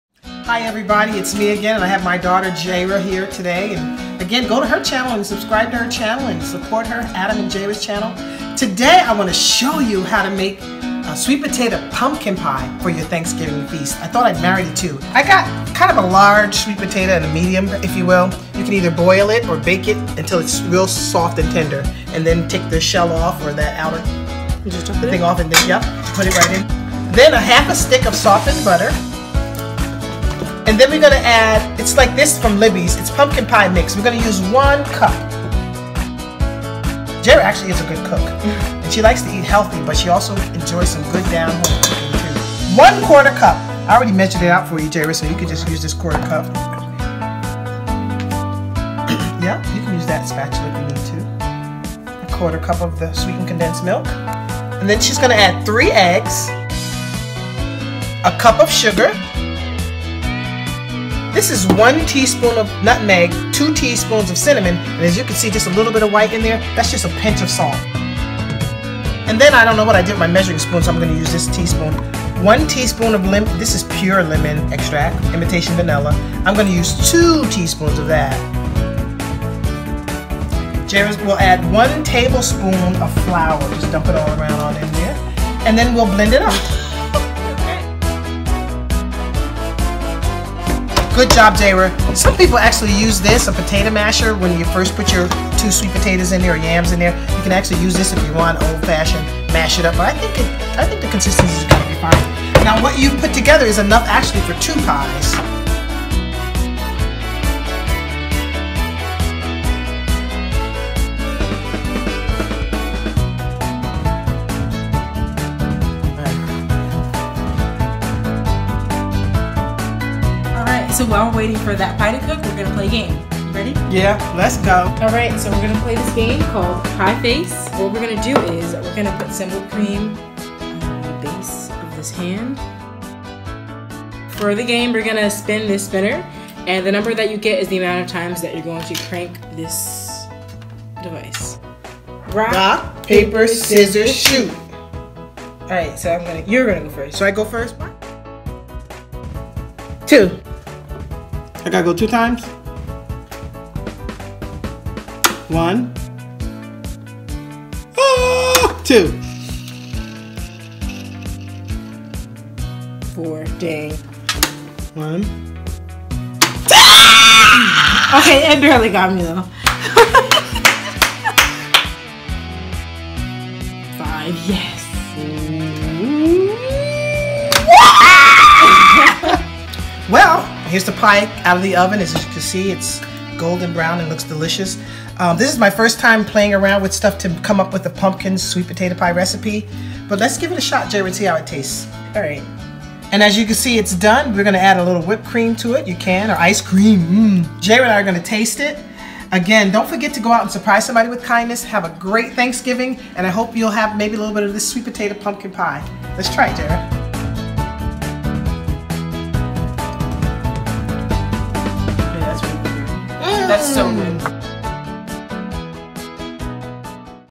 Sweet Potato Pumpkin Pie!
Try this mash-up of the holidays favorite pies, Sweet Potato and Pumpkin Pie! This recipe is super easy and delicious for your dessert table this holiday season. Don't forget to go out and surprise someone with kindness today!